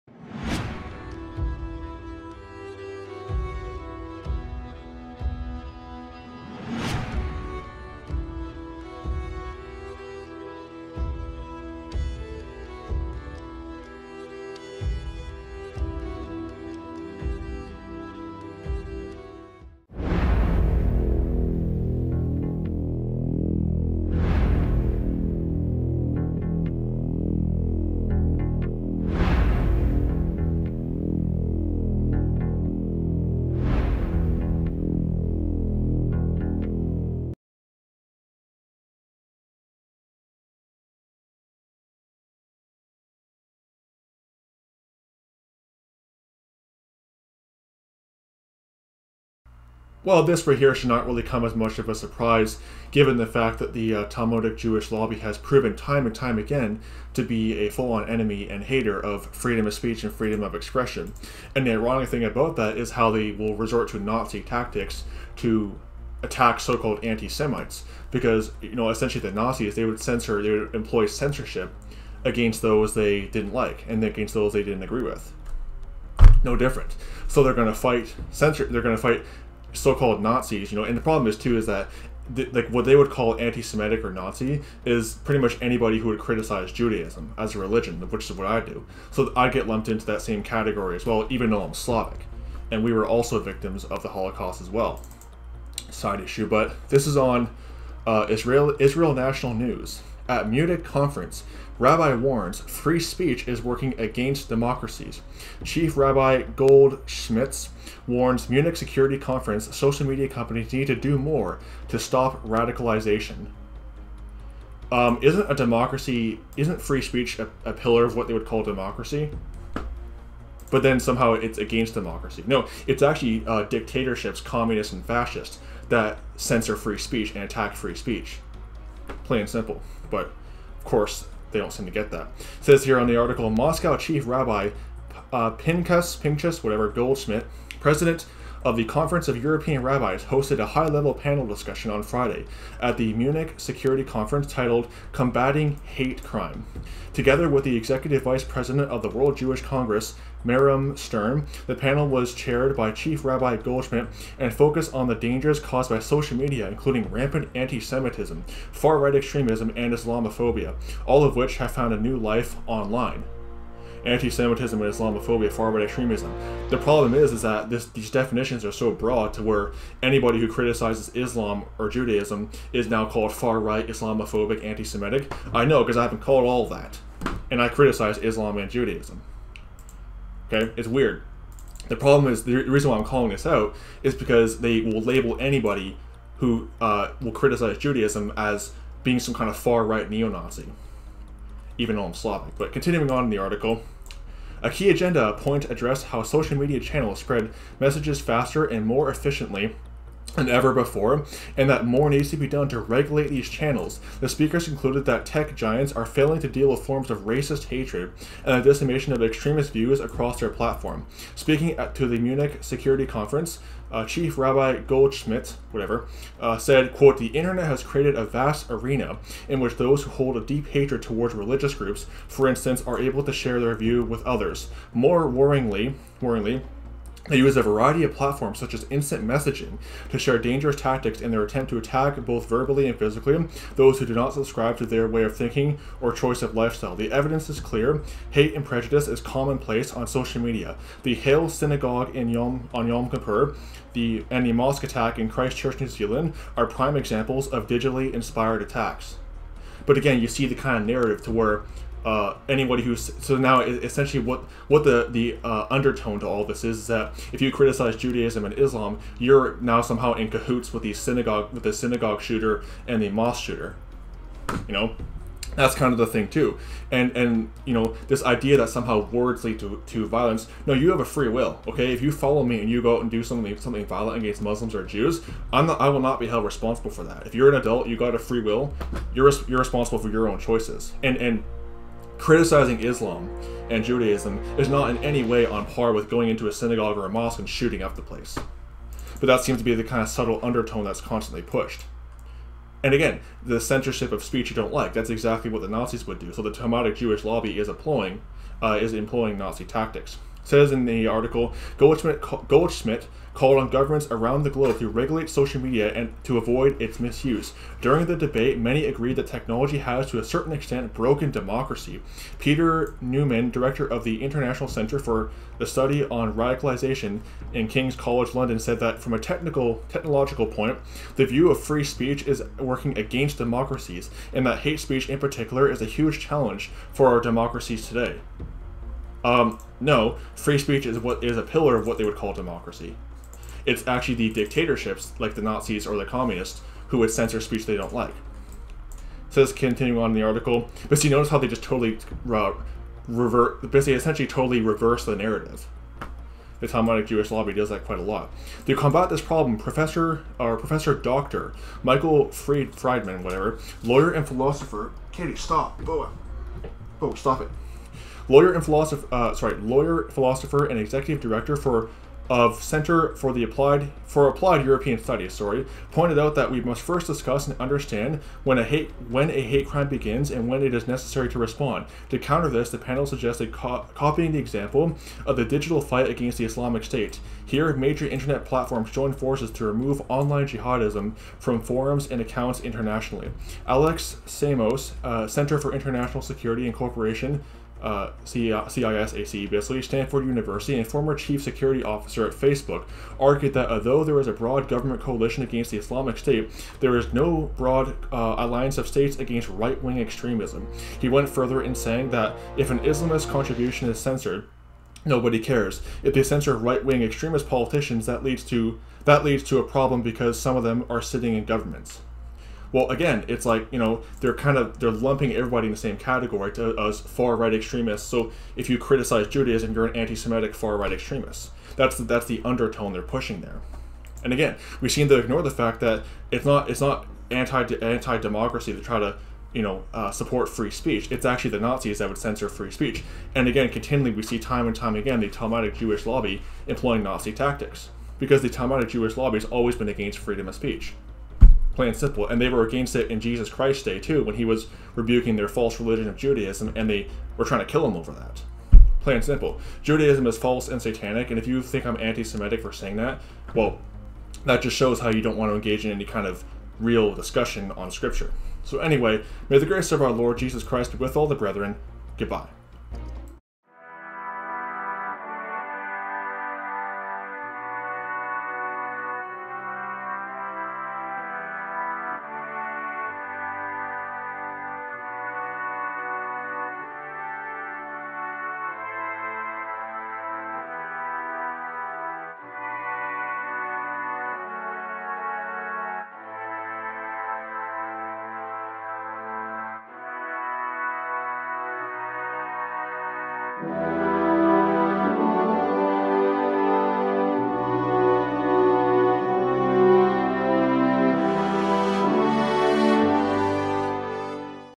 Talmudic Jewish Lobby Wants To Engage In Cyber War Against Free Speech Online
The Talmudic Jewish lobby further proves their hate for free speech by their intent to engage in cyber war against critics of Judaism!